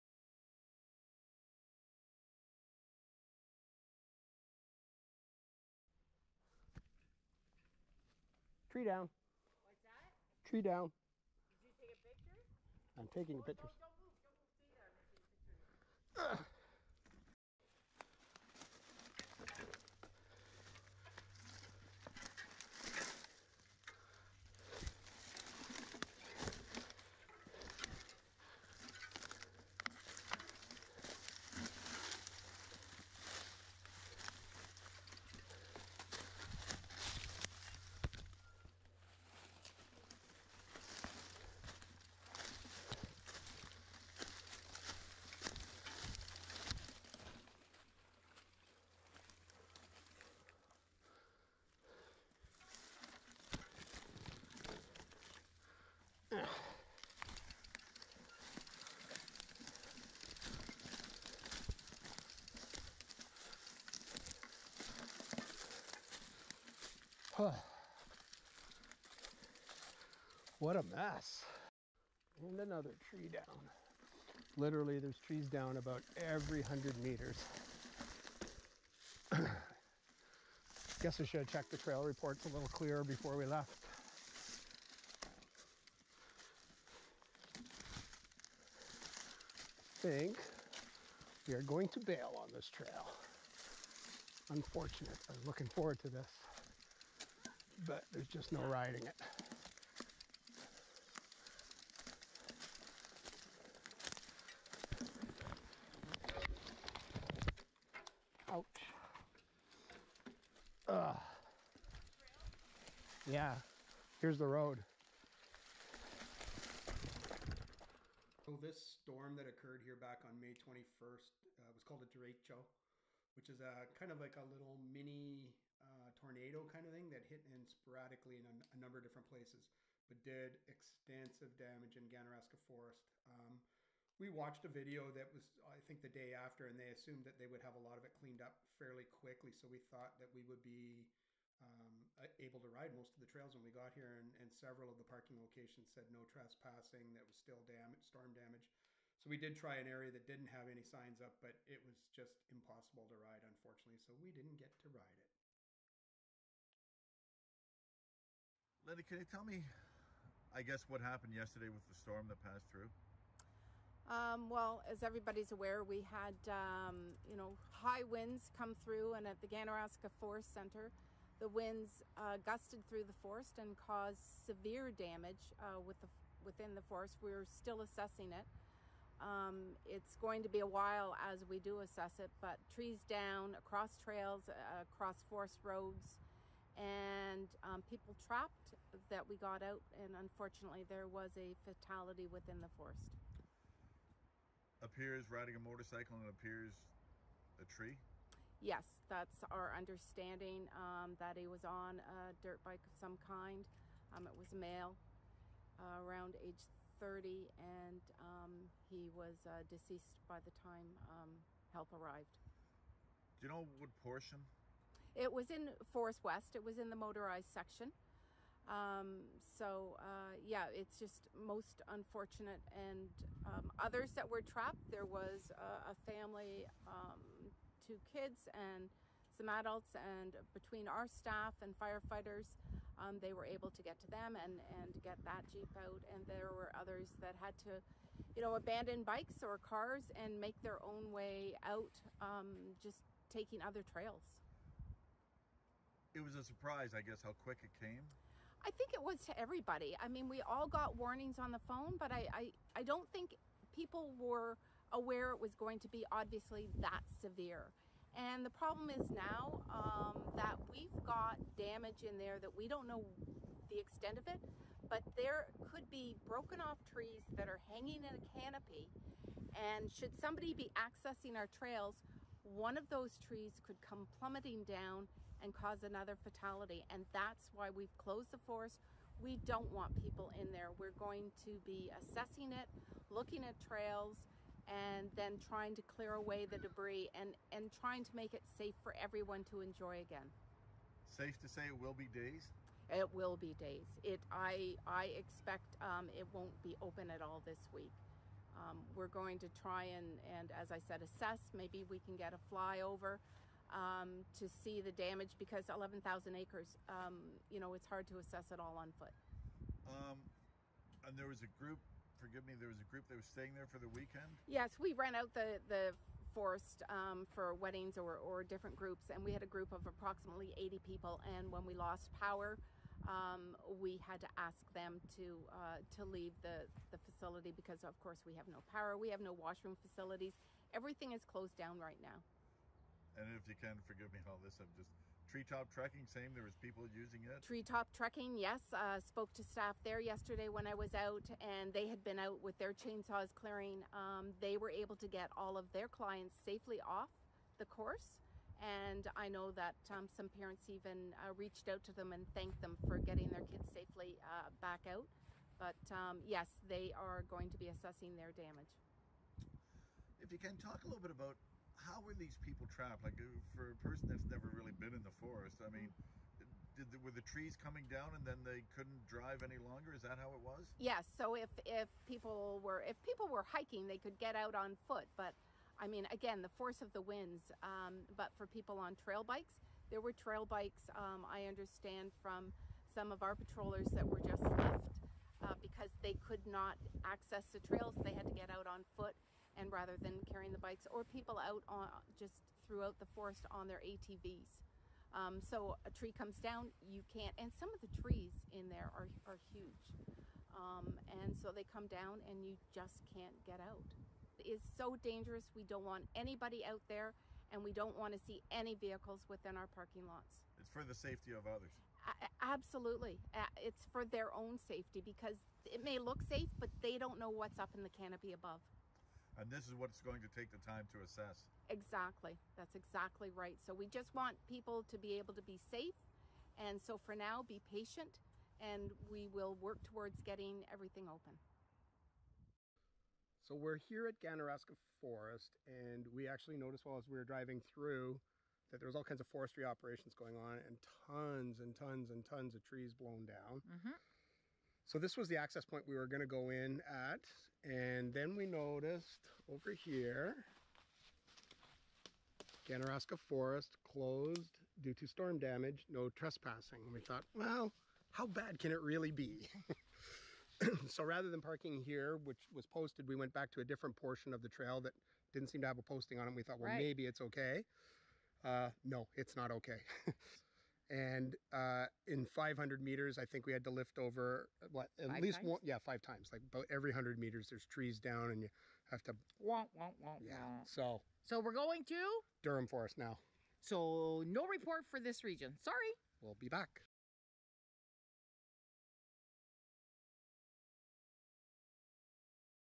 Holy Derecho! Ganaraska Forest Trails were Unrideable!
Welcome to Tourism Region 8: Kawarthas Northumberland. Our visit to the Ganaraska Forest Trails was a bit of a letdown. The storm damage in the Ganaraska Forest on May 21, 2022 was so extensive that they are still working on clearing the trails. The trails were totally unrideable and we will have to come back maybe next year to give them a fair evaluation.